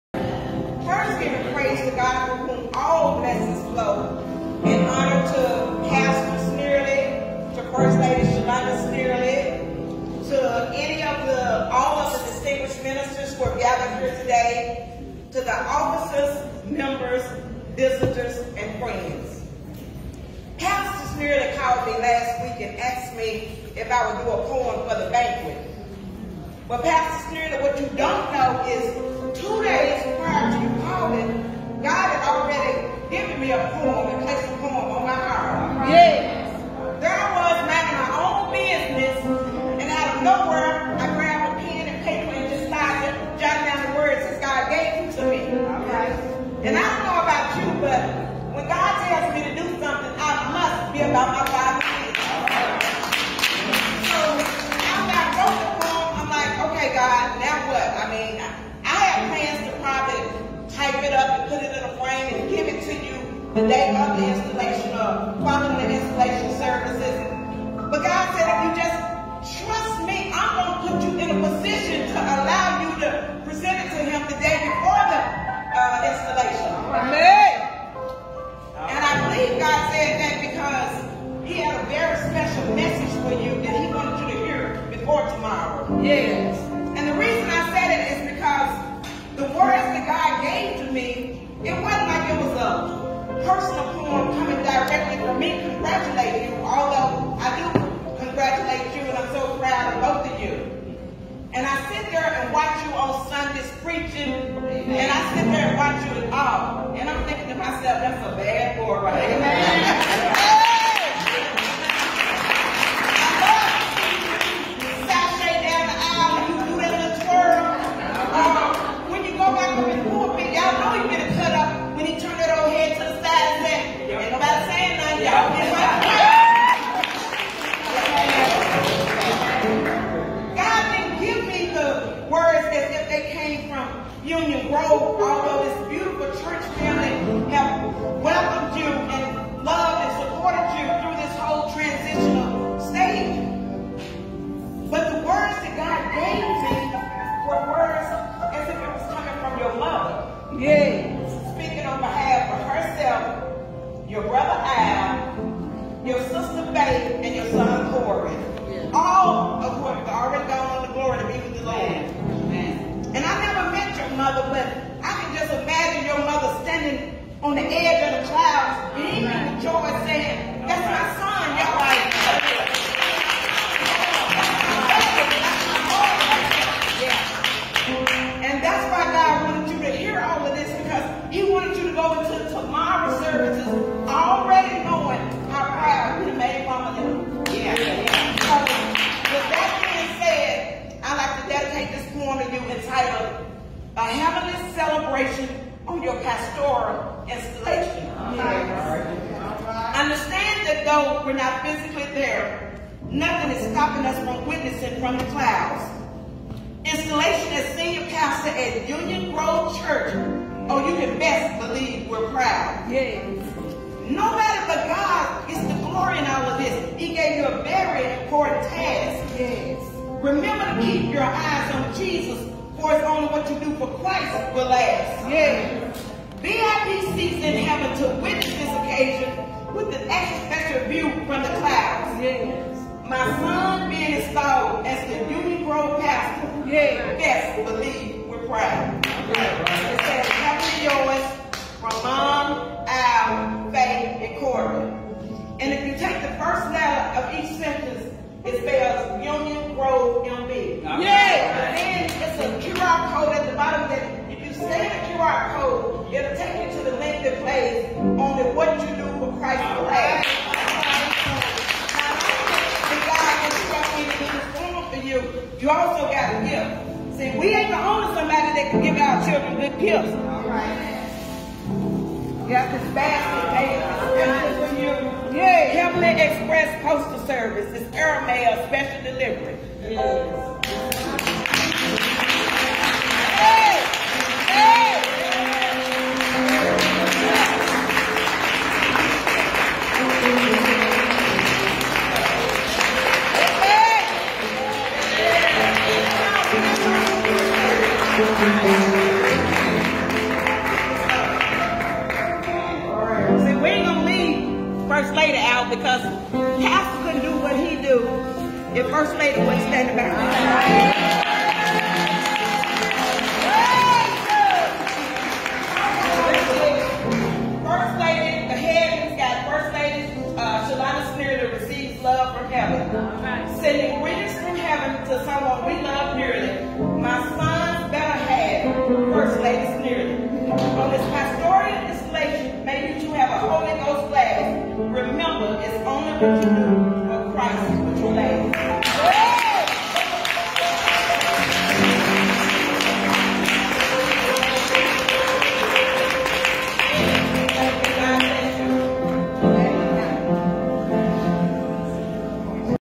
Pastoral Installation Poem- Pastor James Snearley & First Lady Shalanda- 7-15-23 by PoetVelda
This Heavenly Inspired poem was given to me by God to recite at the Pastoral Installation Banquet for Pastor James Snearley & First Lady Shalanda Snearley  of Union Grove Church. Special thanks to my sister Sonya Hunt for capturing the video footage.